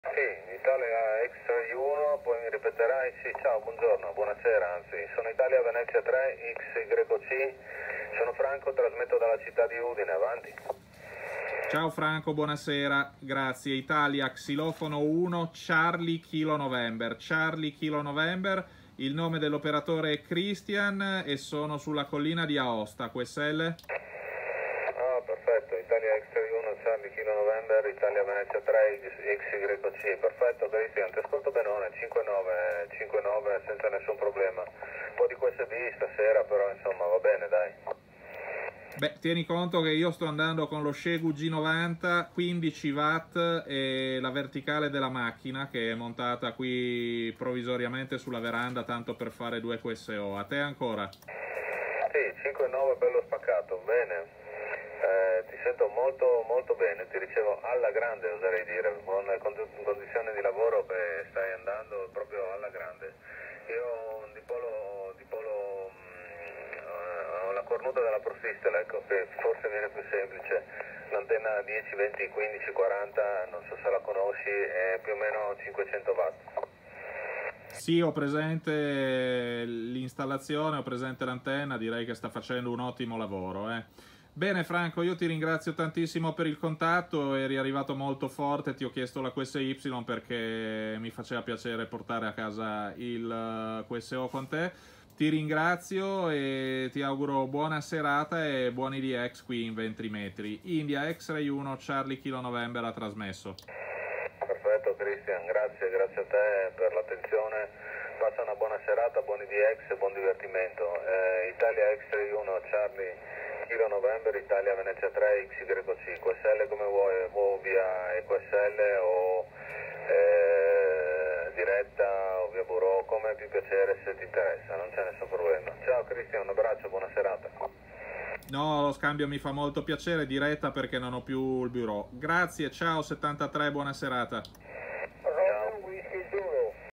14 MHz - Short skip in 20 meters
Contact between Italian amateur radio stations IX1CKN (Chris in Aosta, JN35pr) and IV3XYC (Franco in Udine, JN66nb). Set-up for IX1CKN: Xiegu G90, and mobile antenna Outback HF-1899.